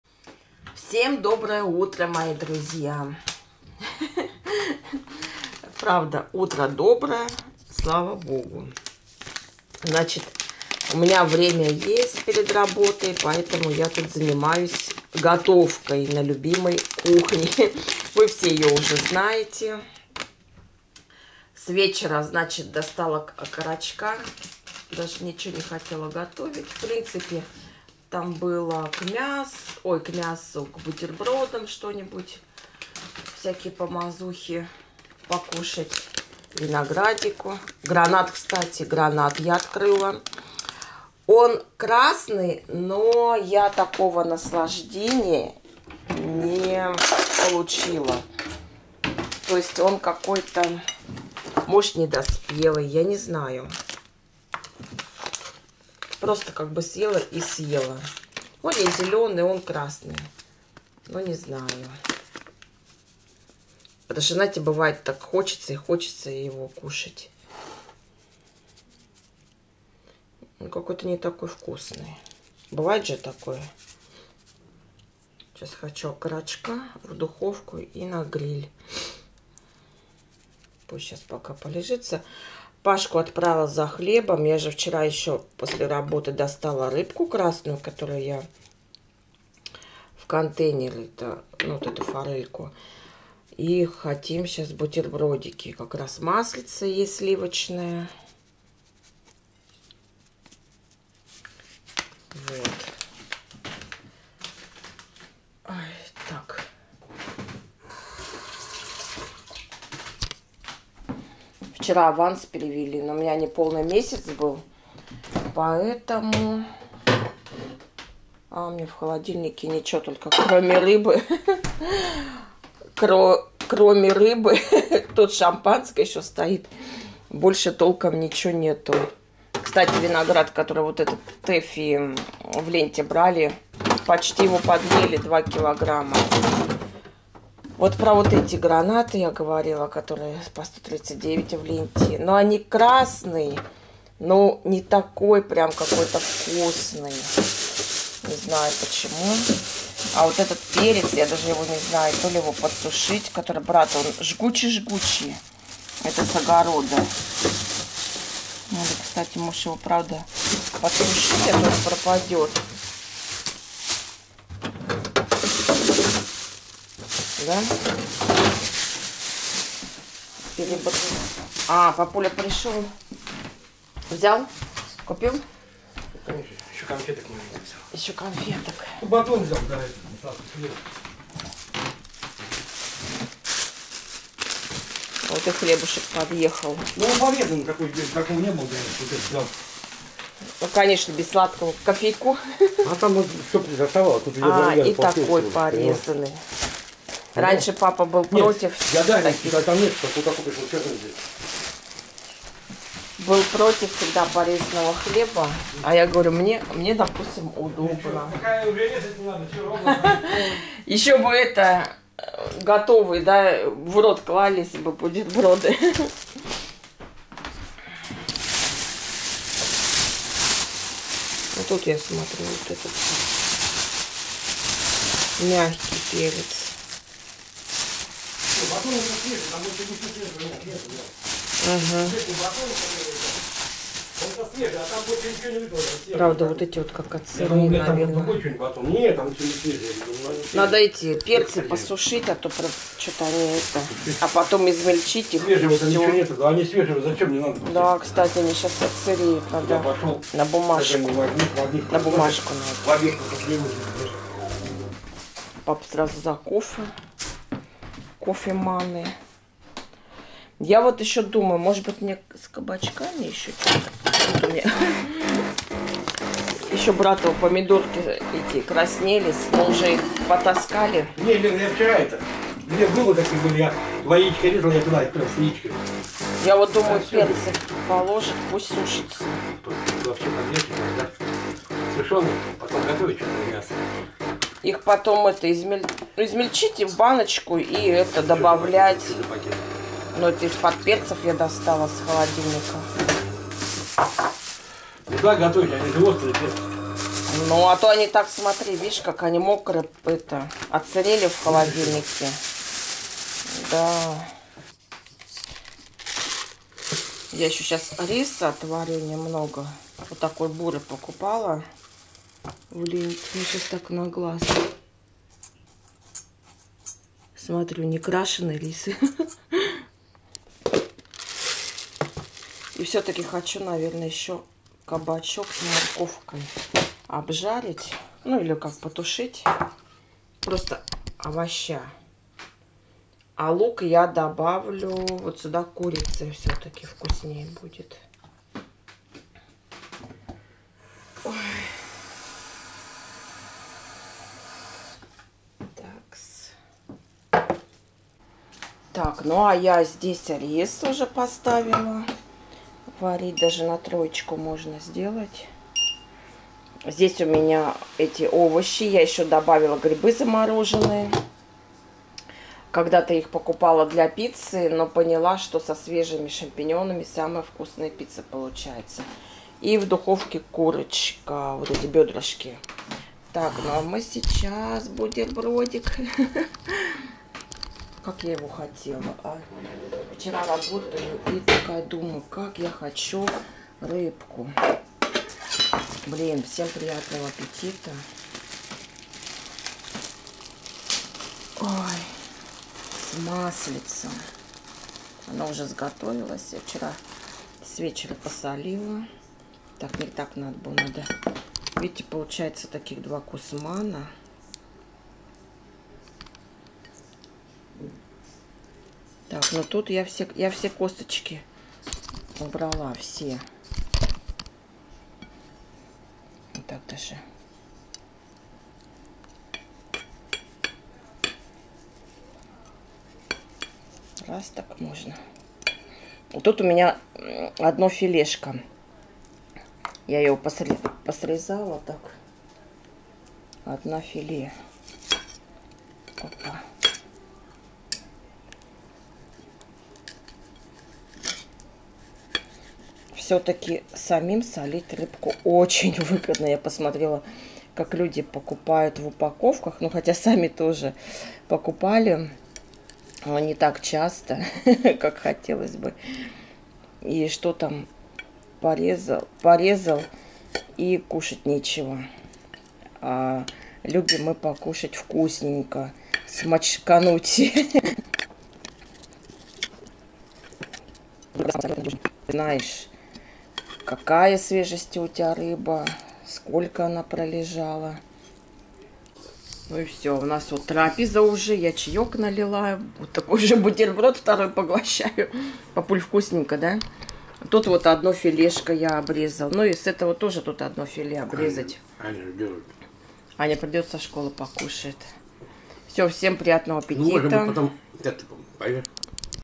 VLOG утром перед работой/завтрак/что приготовить за 30 минут!
Влог. Собираюсь на работу, приготовила на семью, завтракаем.